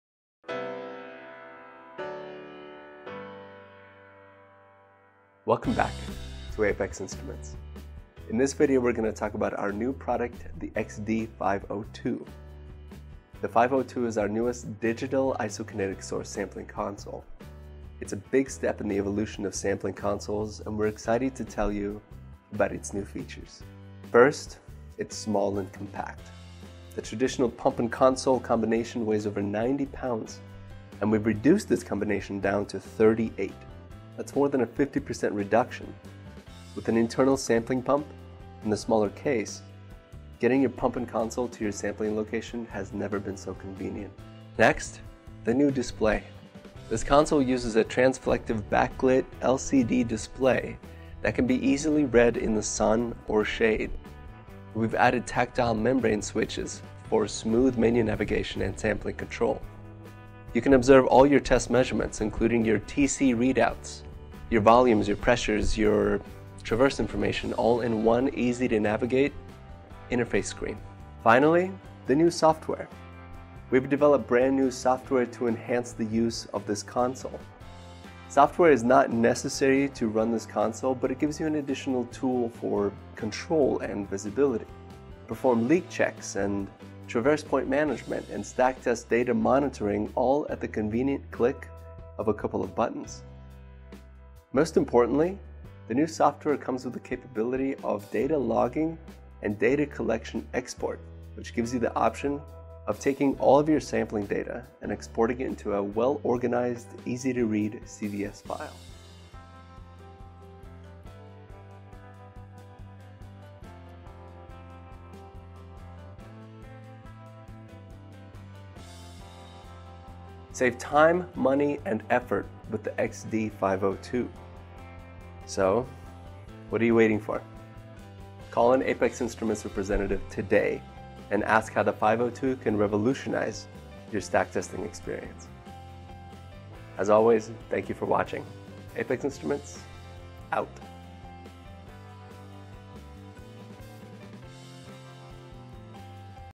XD-502 - Digital Source Sampling Console
The next step in the evolution of digital source sampling.

Some of the exciting features of the XD-502 include an internal sampling pump, a sun-or-shade readable display, and optional software designed specifically for this console.

For additional questions or comments contact an Apex Instruments representative.